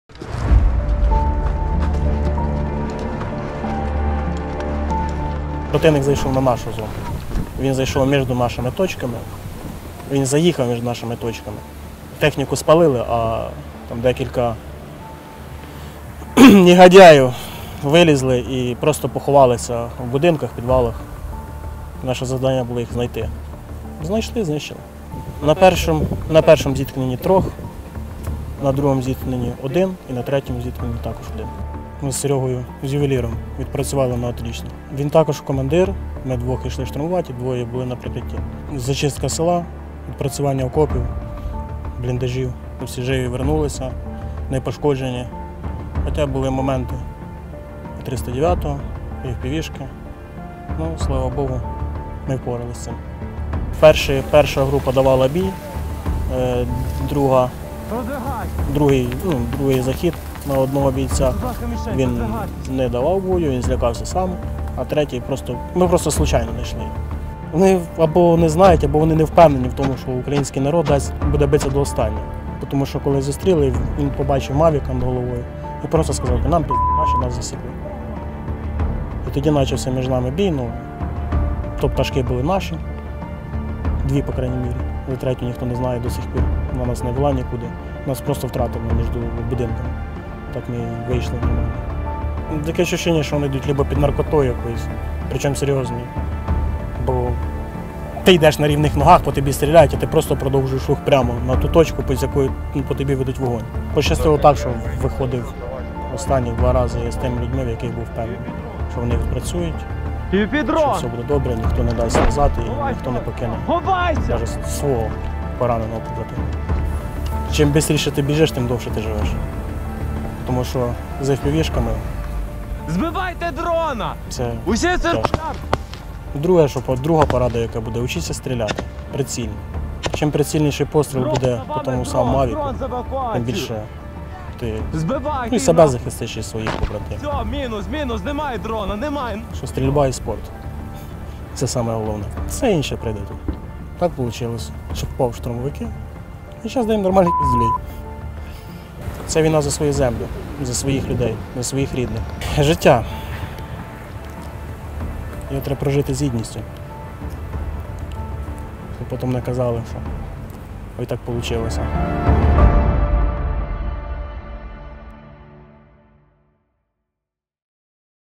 ПОСЛУХАЙТЕ ЦЕ! Росіяни штурмують ПІД НАРКОТИКАМИ. Просто йдуть НА КУЛІ / ДШВ
Командир роти десантників розповів про штурми російських позицій та зачистку територій. За його словами, у військовослужбовців ЗСУ летіло все: міни, артилерія, дрони. Росіяни ж у свою чергу, не боялись йти під кулі і було таке відчуття, ніби вони під наркотиками.